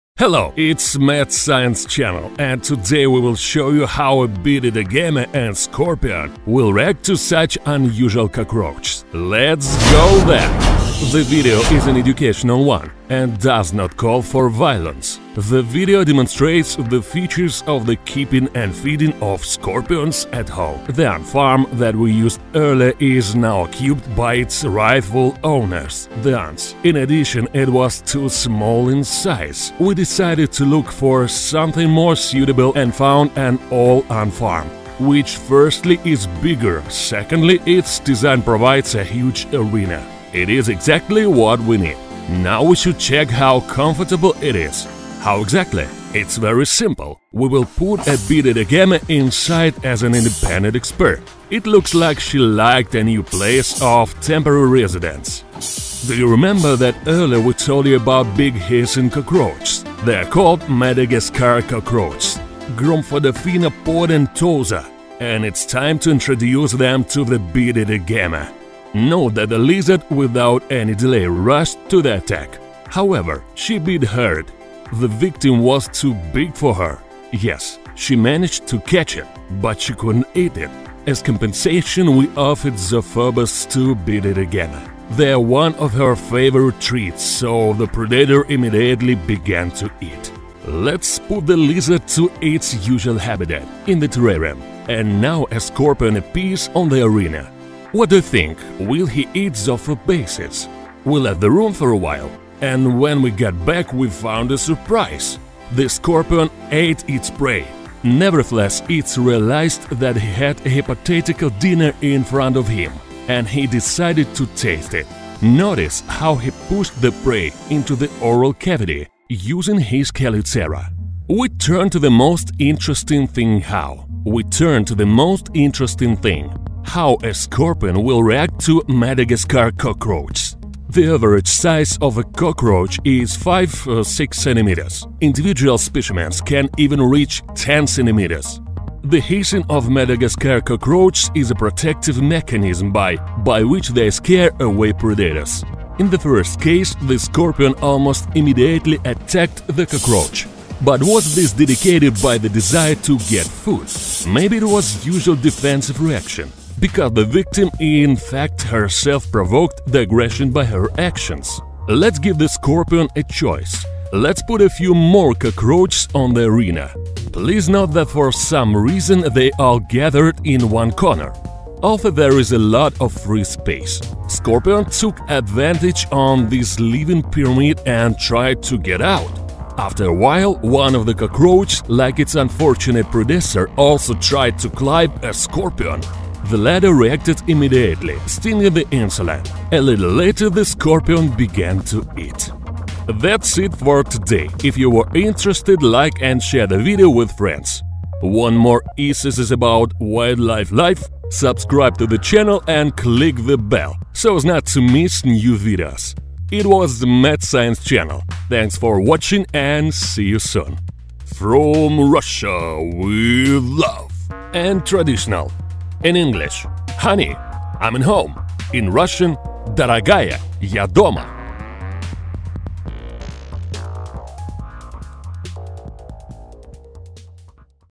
BIG SCORPION AND HUGE HISSING COCKROACHES - THEY EATS HIM WITH AGAMA!
In this video you will see reaction of a scorpion to the big hissing cockroaches. As you know - a scorpion a predator - and eats other insects - that will occur further - you will see in this video! This video does not call for violence! Process of feeding of a scorpion and a bearded agama is shown in video!